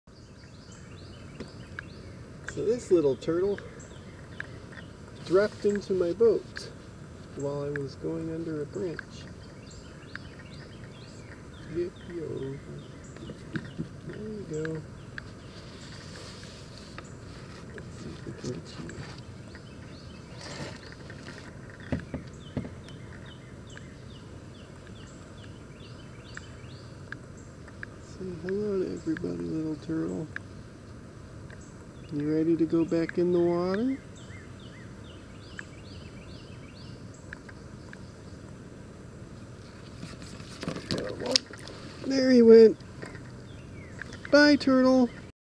Paratrooper Turtle on Bayou Fountain
This little turtle dropped from a branch into my canoe as I was paddling Bayou Fountain.  Once he got his bearings, I gave him a lift back to safety.  (First reptile ever to drop into my canoe on the bayou.)